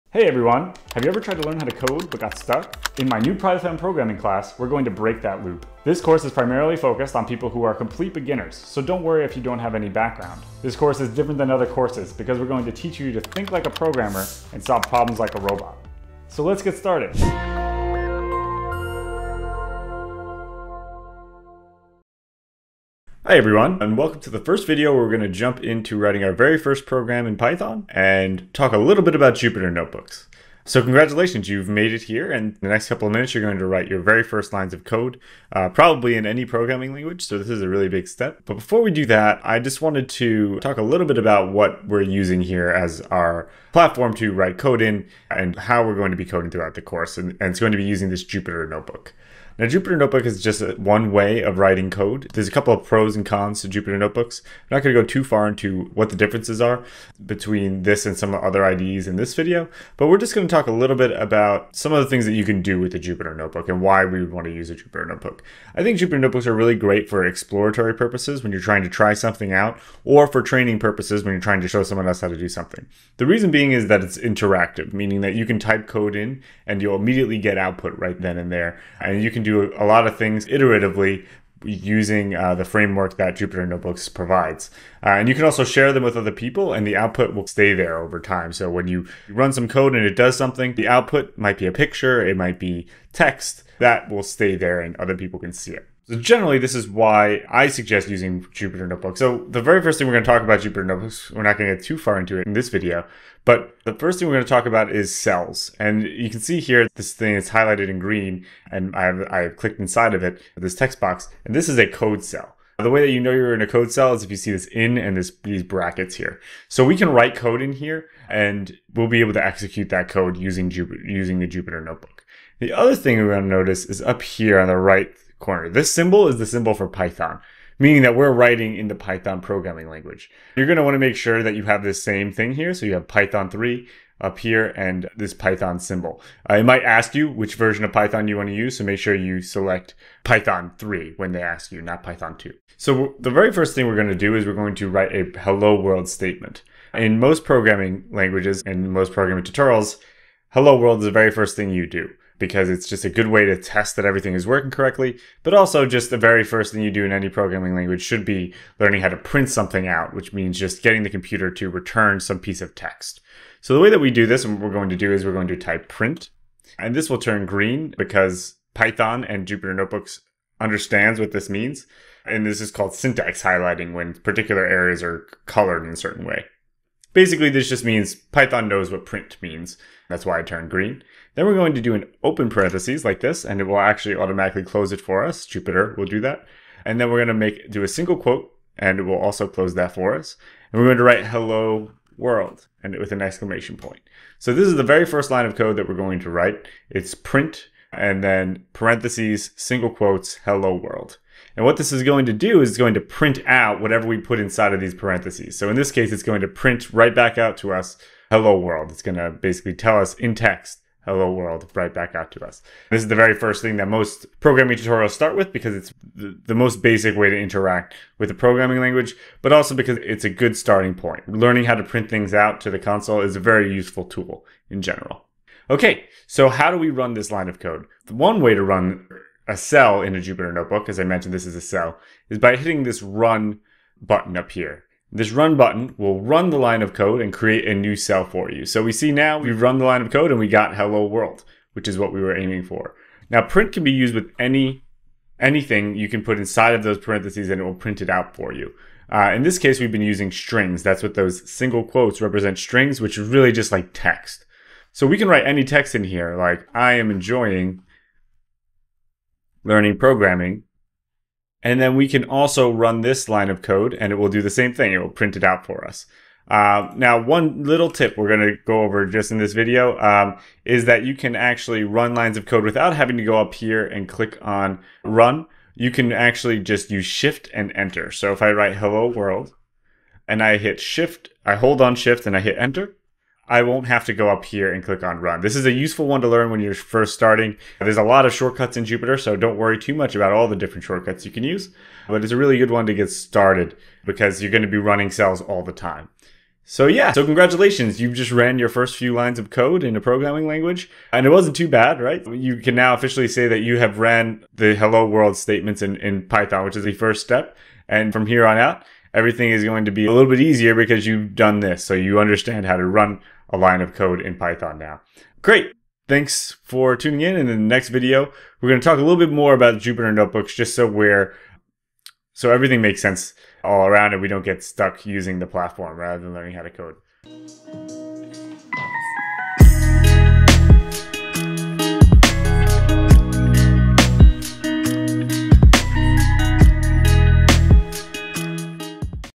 Hello World in Python: Your First Code in Jupyter Notebooks! | Think Like a Robot | Lesson 1 Video 1
This is the first video in the first lesson of the "Think Like A Robot Course"

Welcome to our first actual coding video in Think Like a Robot! Dive into Python programming with us as we explore the basics in Jupyter Notebooks. We discuss the advantages of using Jupyter, focusing on its interactivity and suitability for exploration and training. Follow along as we write and execute our inaugural code—a classic "Hello World" statement. Learn about code cells, syntax highlighting, and running cells effortlessly with shortcuts. Congratulations on running your first lines of code! Join us for more insights into Jupyter Notebooks in the next video, ensuring a smooth coding journey ahead.

Note that videos in this course are being released every 2-4 days, so please be patient as new videos come out.

Thank you for joining us on this exciting programming adventure!